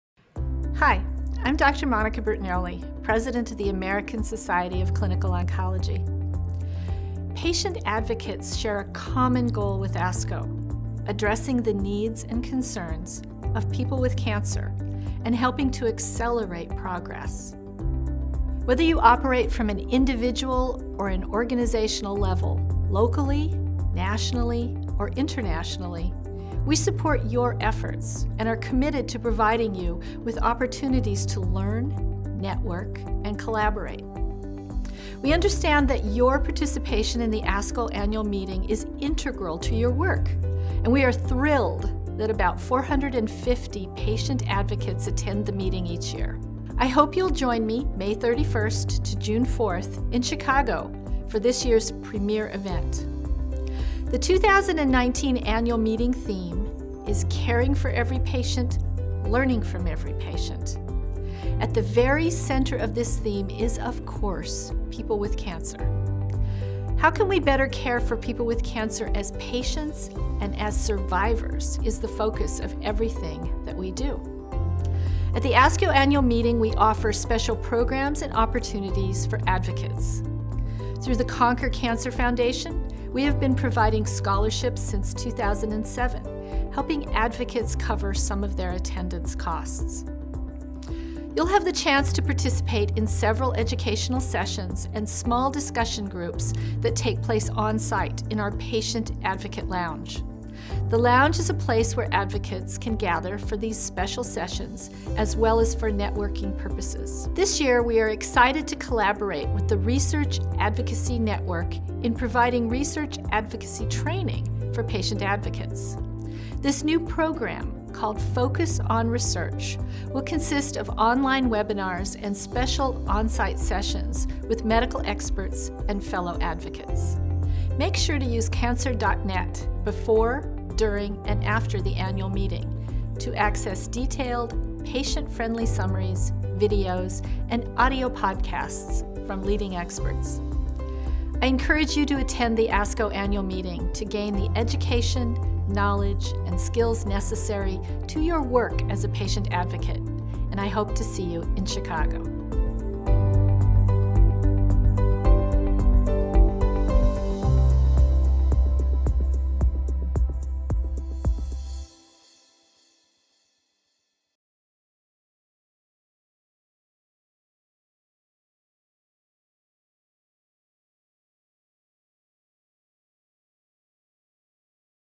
ASCO Annual Meeting Invitation for Patient Advocates
Watch this video from ASCO President Monica M. Bertagnolli, MD, FACS, FASCO to learn about why ASCO2019 is a must-attend conference for patient advocates.